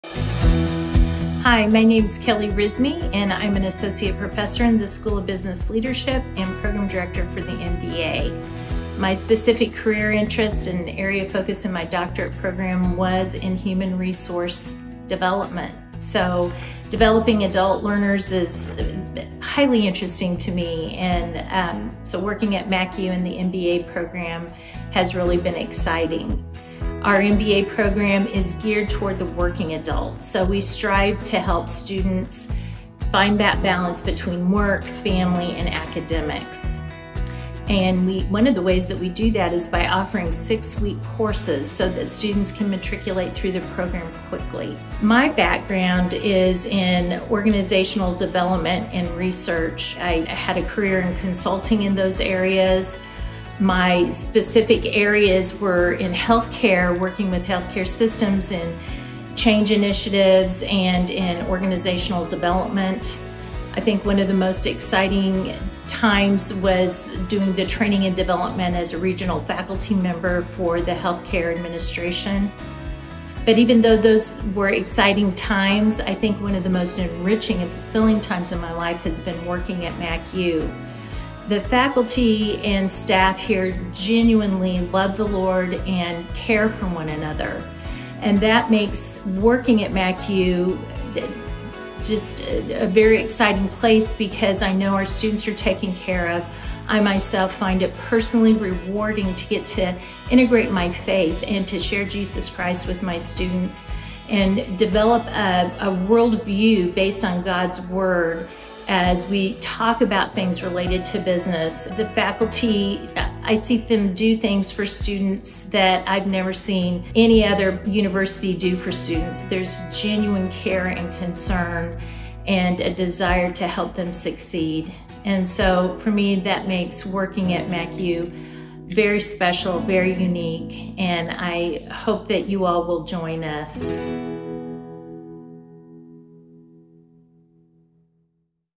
MACU Meet Your Professor: Dr. Kelly Riesenmy
Dr. Kelly Riesenmy is dedicated to developing adult learners in MACU's esteemed School of Business Leadership. With a background in business, human resources, and healthcare administration, Dr. Riesenmy brings a rich classroom experience to her adult students.

Hi, my name is Kelly Riesenmy and I'm an associate professor in the School of Business Leadership and program director for the MBA. My specific career interest and area of focus in my doctorate program was in human resource development. So developing adult learners is highly interesting to me, working at MACU in the MBA program has really been exciting.

Our MBA program is geared toward the working adult, so we strive to help students find that balance between work, family, and athletics. One of the way we do that is by offering 6-week courses so that students can matriculate through the program quickly. My background is in organizational development and research. I had a career in consulting in those areas, my specific areas were in healthcare working with healthcare systems and change initiatives and in organizational development.

I think one of the most exciting times was doing the training and development for as a regional faculty member for the healthcare administration. But even though those were exciting times, I think one of the most enriching and fulfilling times in my life has been working at MACU. The faculty and staff here genuinely love the Lord and care for one another and that makes working at MACU just a very exciting place because I know our students are taken care of. I myself find it personally rewarding to get to integrate my faith and to share Jesus Christ with my students and develop a worldview based on God's Word as we talk about things related to business. The faculty, I see them do things for students that I have never seen any other university do for students. There is genuine care and concern and a desire for them to succeed. So for me that makes working for MACU very special and unique and I hope that you all will join us.